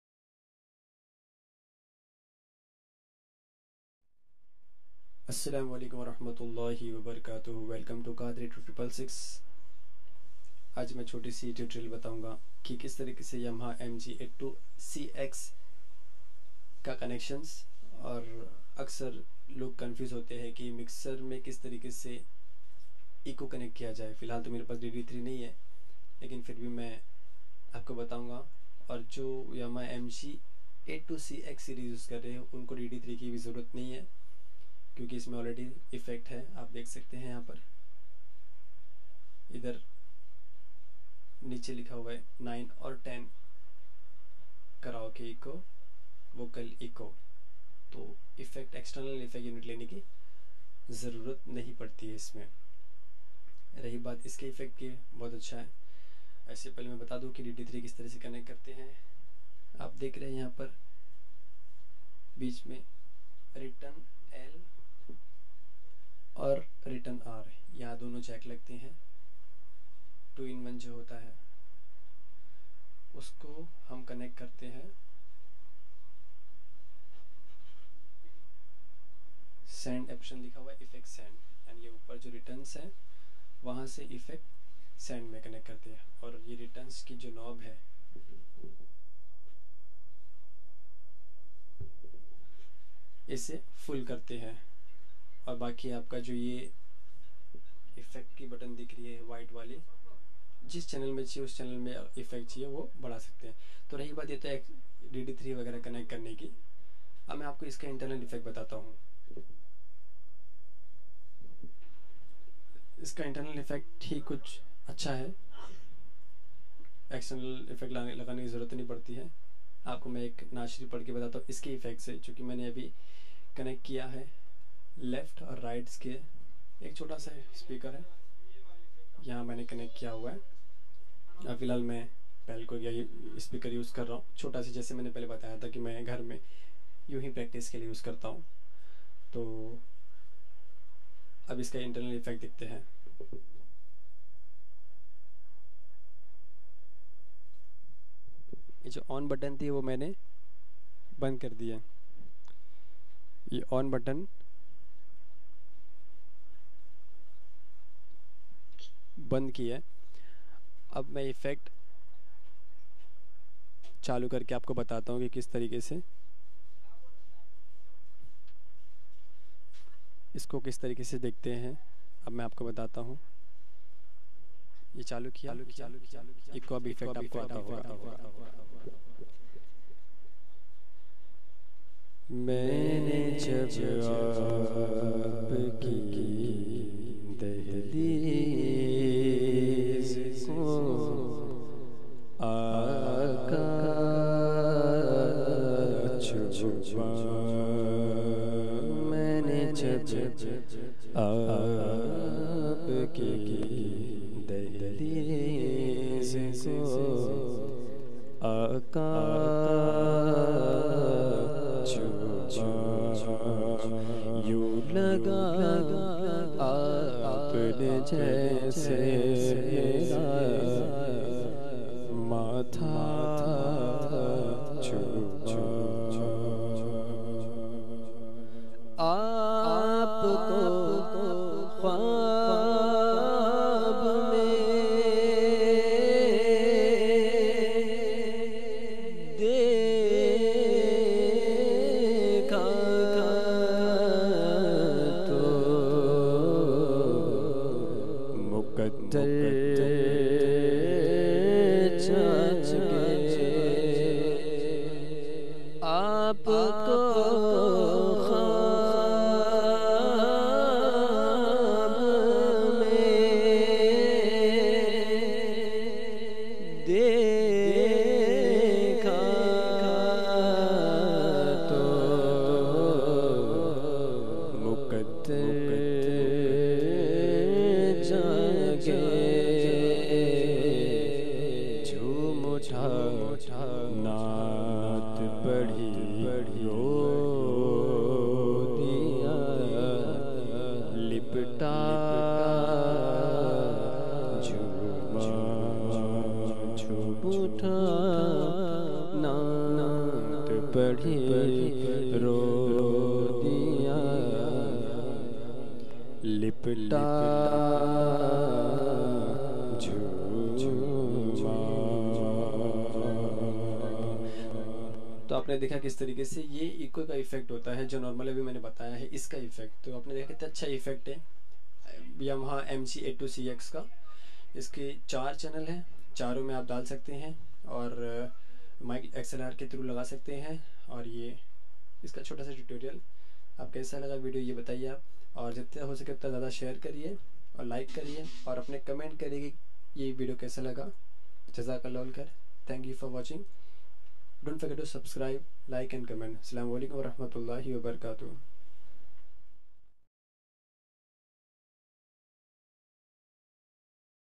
Eco setting in Mixer Yamaha MG82CX in Naatsharif
Eco setup in Mixer which don't require extra effect unit for vocalist perfect for live sound and studio recording. Extremely Sorry i forgot to set the pan due to which the volume is only in left Channel